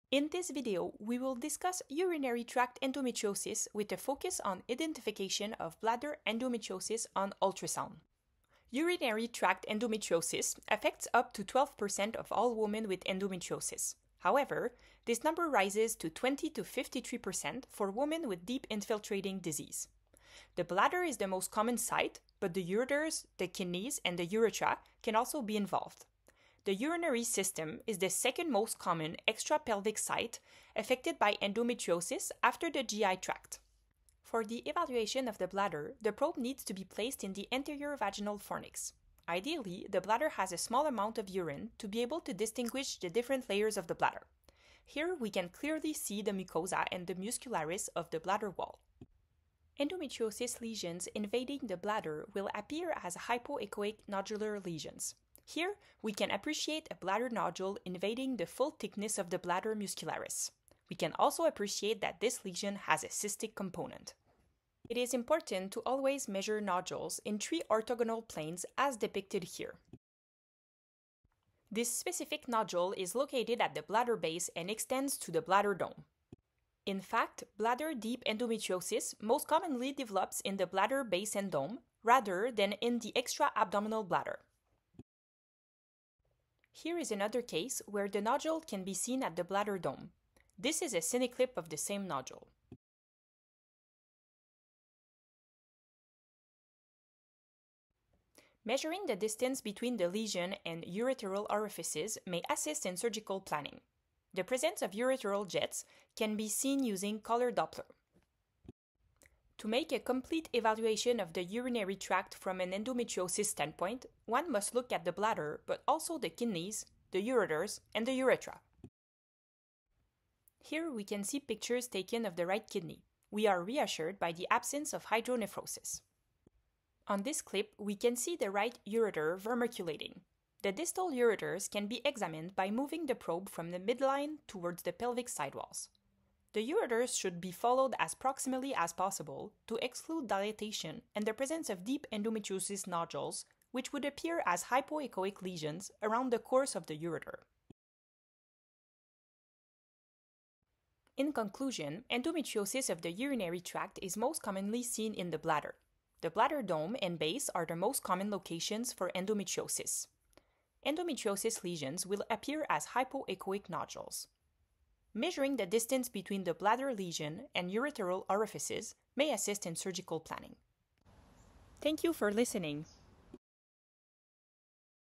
Urinary Tract Endometriosis: Identification of Bladder Endometriosis on Ultrasound
This short video is a quick way to learn about bladder endometriosis. We take you through visualizing the normal bladder and normal ureters on ultrasound, how to measure bladder endometriosis, and other important surgical principles such as the bladder-to-ureteral orifice distance.

Our video uses the terminology "women" but we do recognize endometriosis can affect anyone assigned female at birth, regardless of their gender identity.

The video is voiced by current MIGS/ultrasound fellow Dr Mélissa Marien.